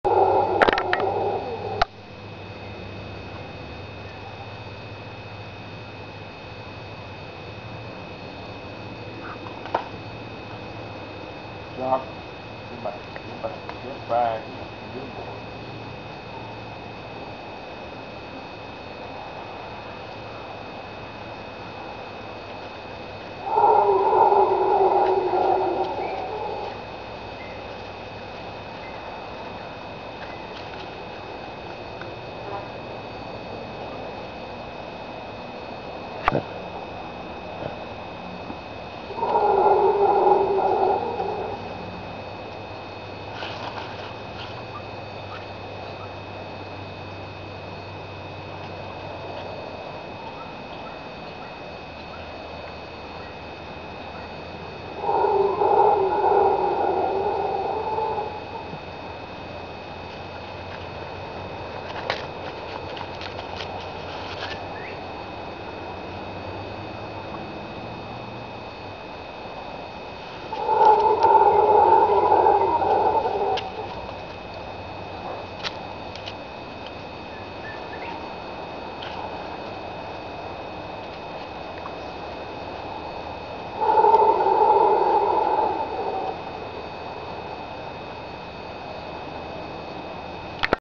howler monkeys
Howler monkeys from the forests surrounding Mombacho Volcano come within 300 yards of our rental every evening lokking for water. Here's how they sound...